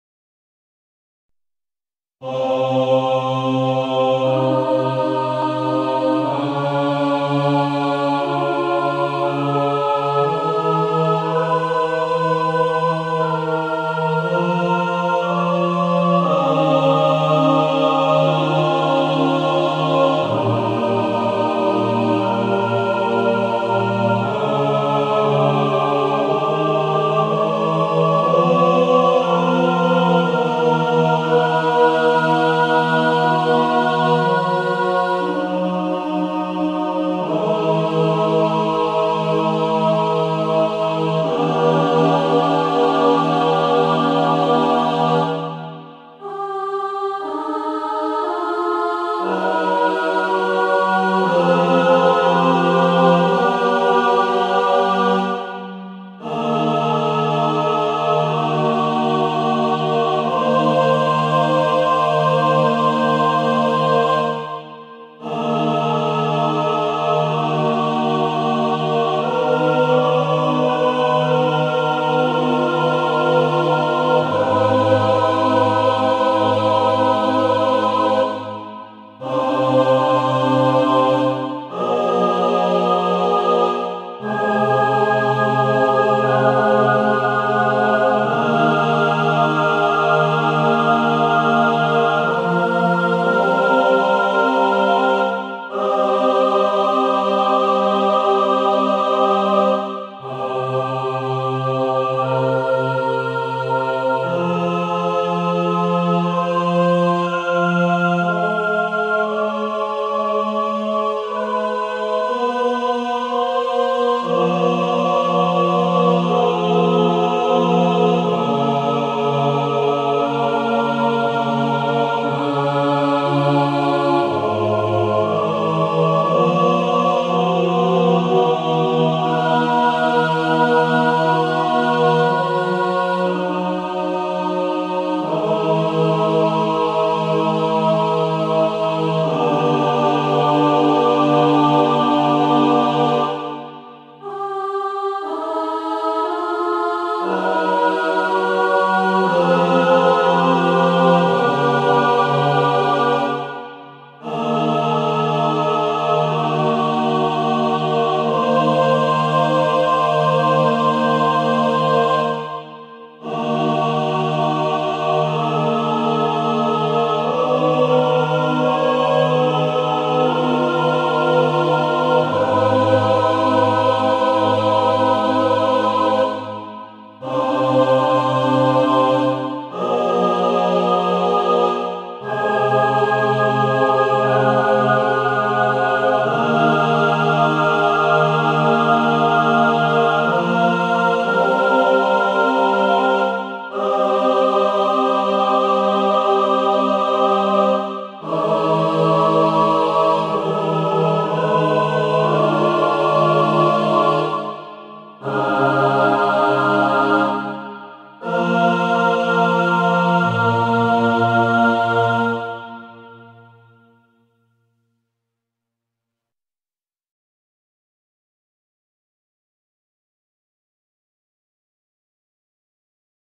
a choral study in 17 equal - animated score
middle C is in common between this tuning od 17 steps per octave and 12 steps per octave.